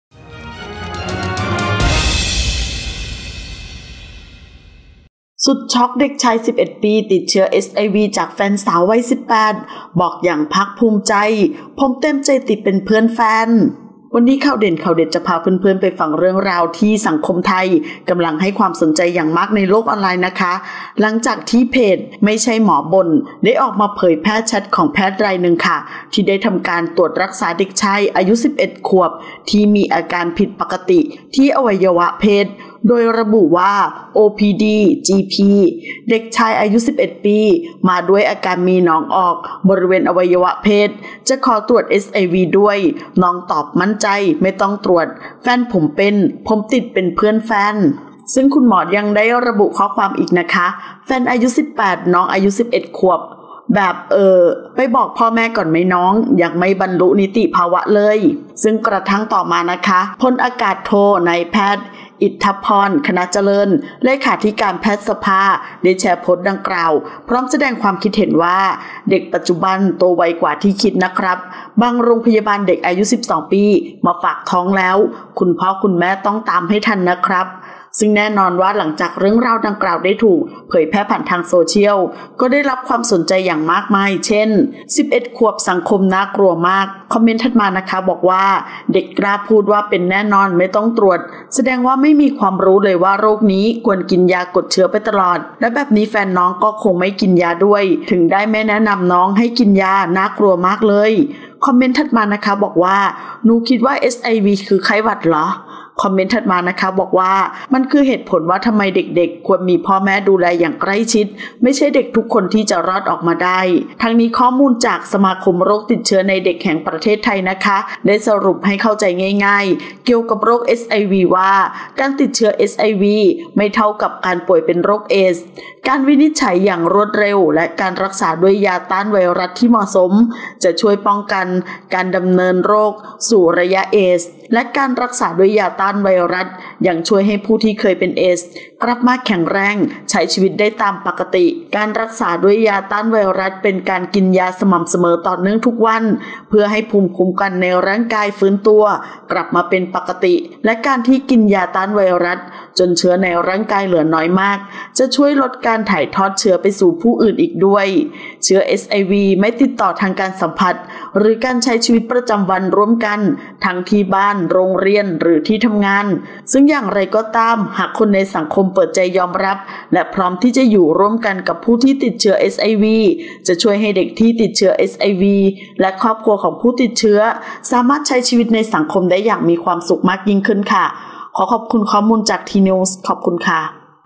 สุดช็อก! ด.ช.วัย11 ติดเชื้อ HIV จากแฟนสาววัย18 บอกอย่างภาคภูมิใจ “ผมเต็มใจติดเป็นเพื่อนแฟน” : RECAP
สุดช็อก! ด.ช.วัย 11 ติดเชื้อ HIV จากแฟนสาววัย 18 บอกอย่างภาคภูมิใจ “ผมเต็มใจติดเป็นเพื่อนแฟน” : RECAP

00:00 - Intro
00:10 - News
01:00 - Minute